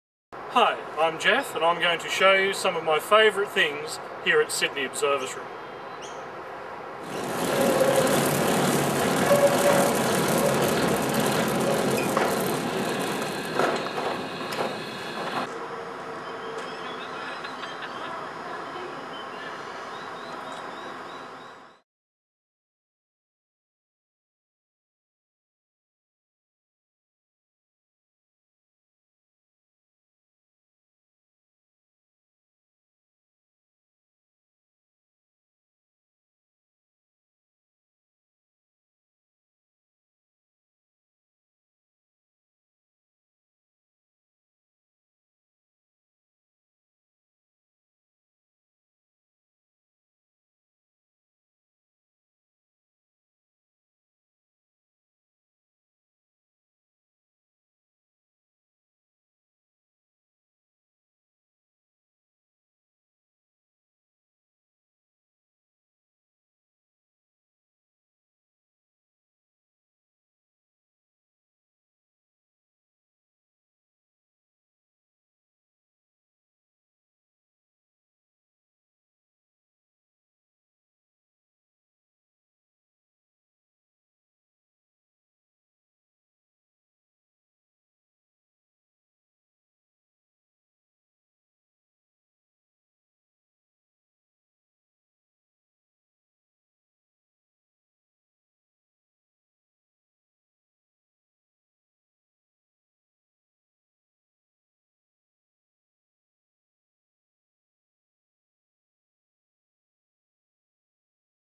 Sydney Observatory images of the Moon
Video of the Moon and Saturn. Oops, seems I should study my video more carefully! The flooded crater I named as Aristillus is in fact Archimedes which is 83km wide and 2150m deep. I will be more careful next time :(
Regards
Geoff